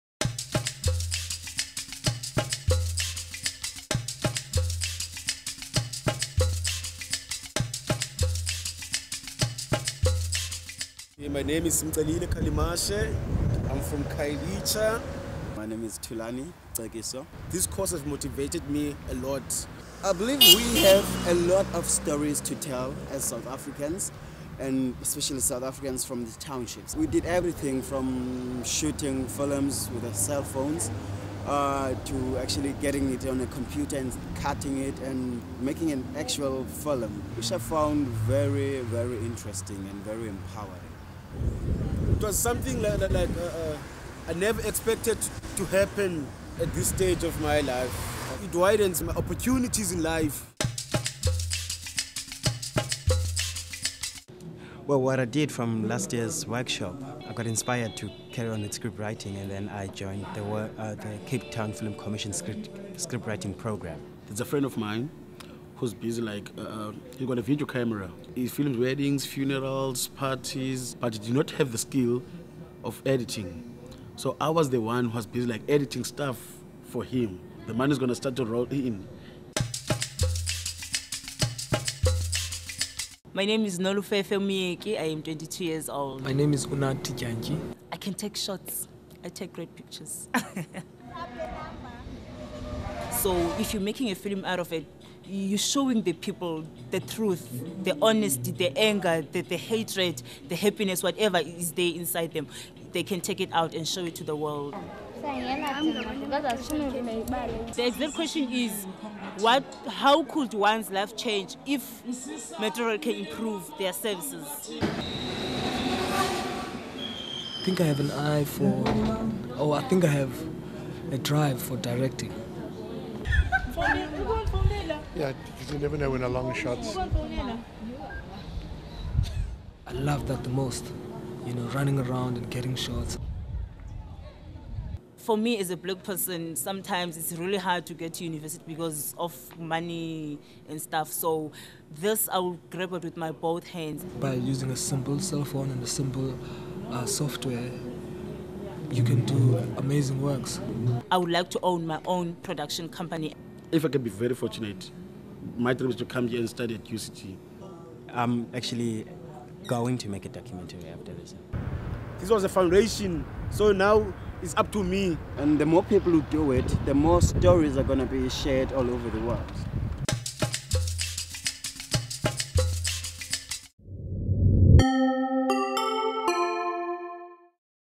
Inside NATA 2
We returned to Cape Town in June, 2009 to deliver a Level 2 Documentary Workshop to students from the New Africa Theatre Association. Several of the participants have been putting their new filmmaking skills to good use since we last saw them. Find out what they have been up to and watch as they create two documentaries about their community!